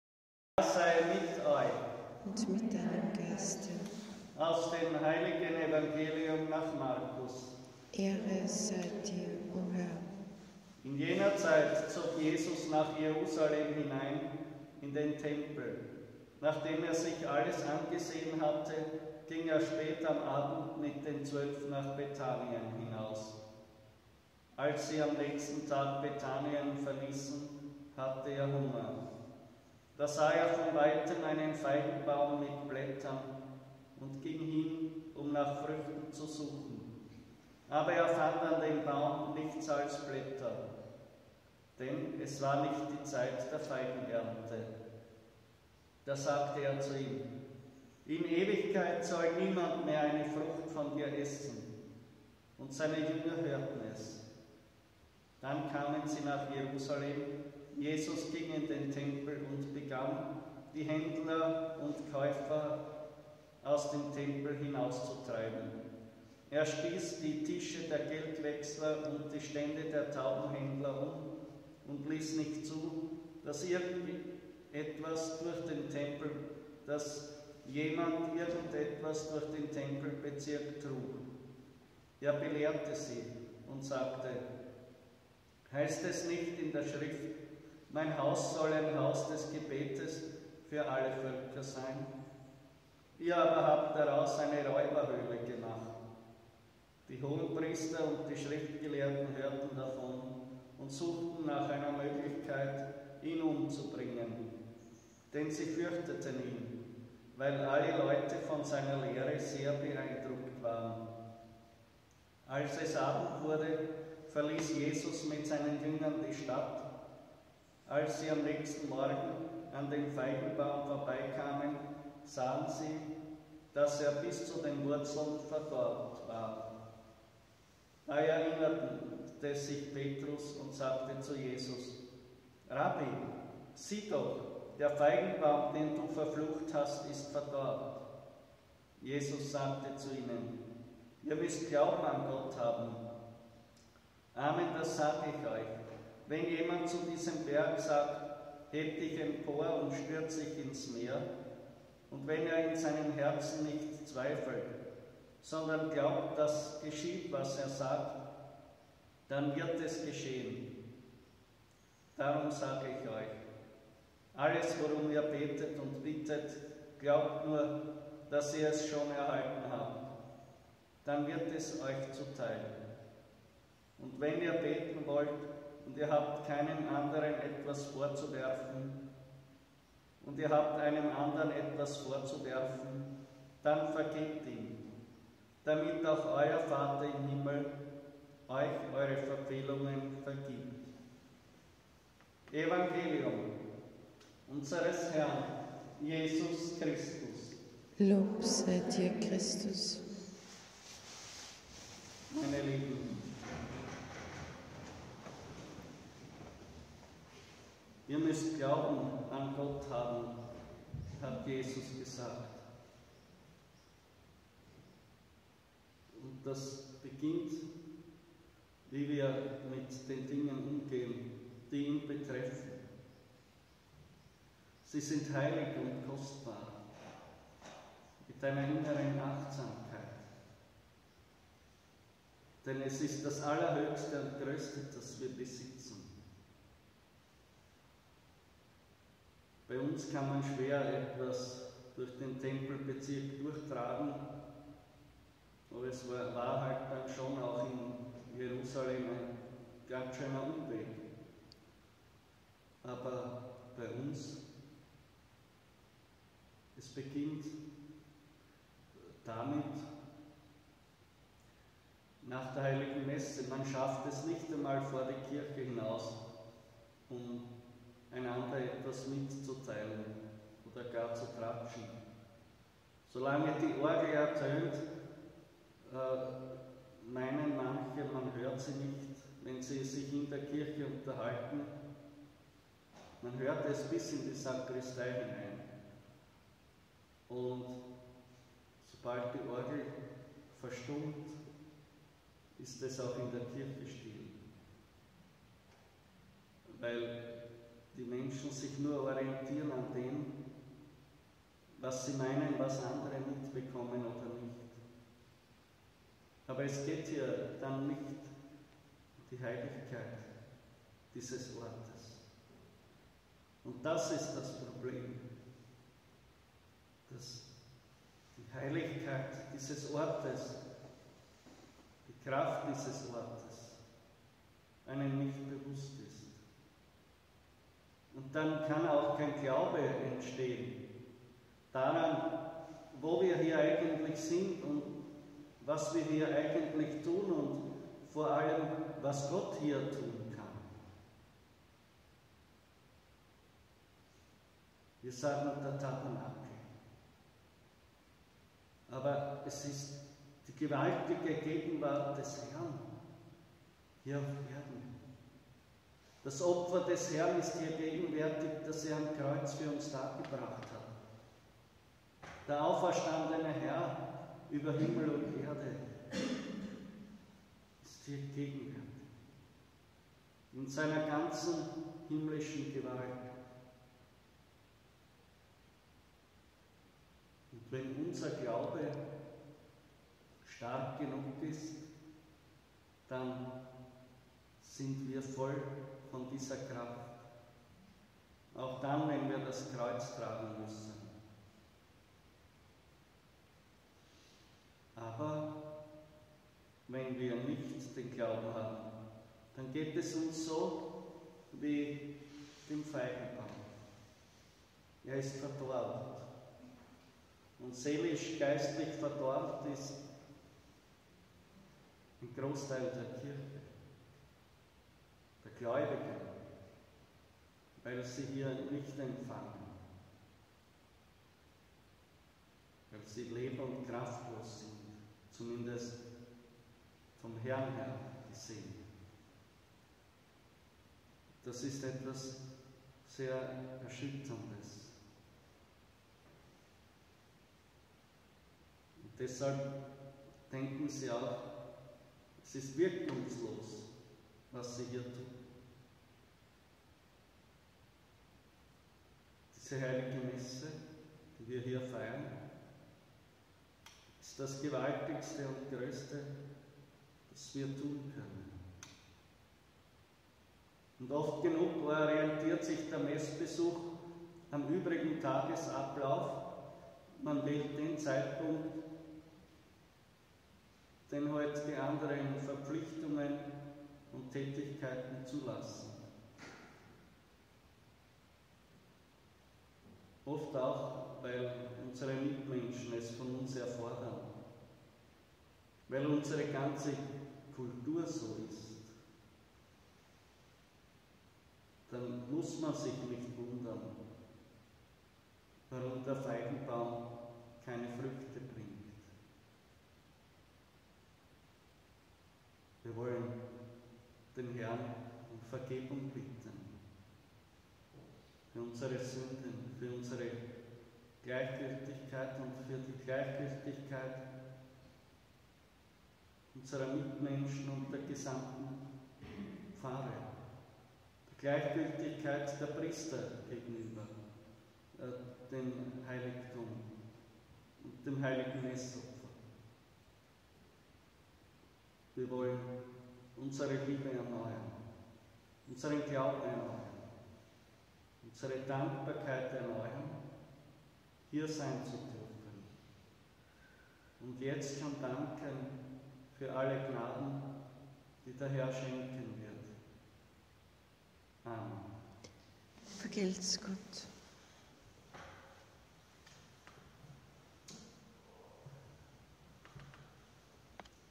Ein Großteil der Kirche ist seelisch und geistlich verdorrt!
HL. EVANGELIUM + Predigt am Freitag der 8. Woche im Jahreskreis - 28. Mai A. D. 2021